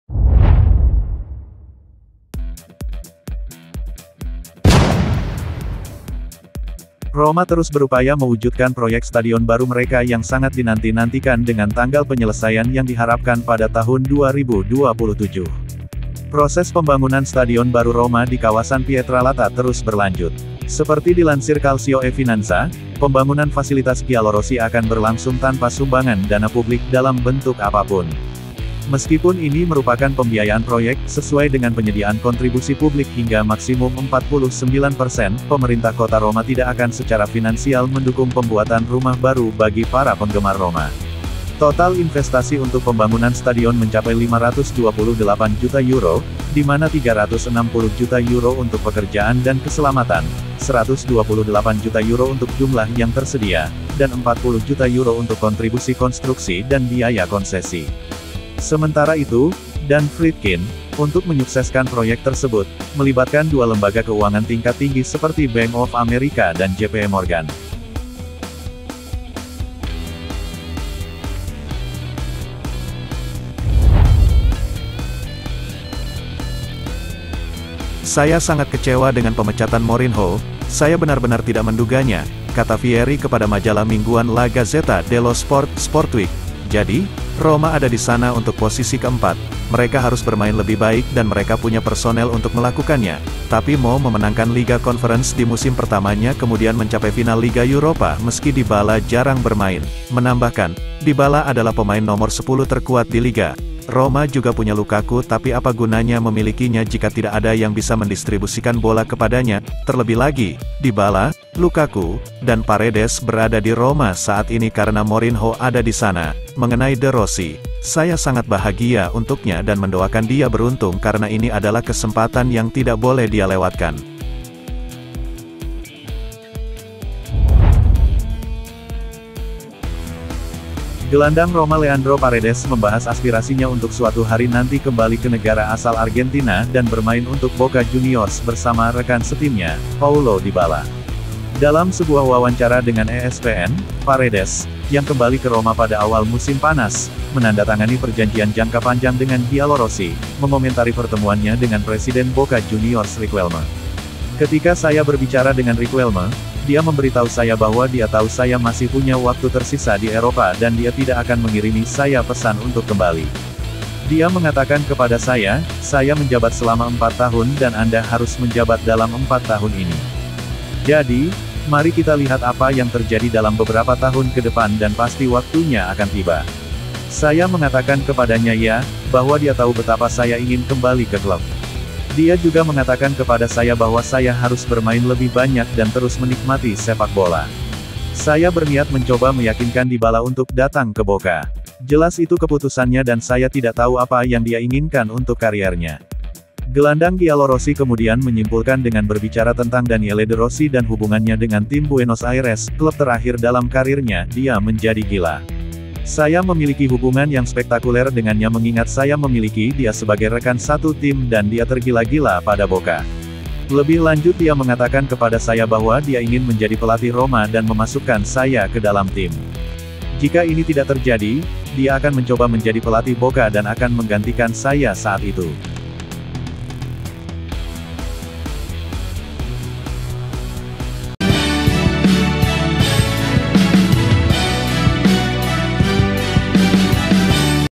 Berita AS Roma hari ini 🔥 Polemik stadion baru AS Roma 🔥 Paredes ingin ke Boca🔥 Vieri kritik Roma
Cristian Vieri kritik AS Roma, Paredes ingin ke Boca, Polemik pembangunan stadion baru AS Roma.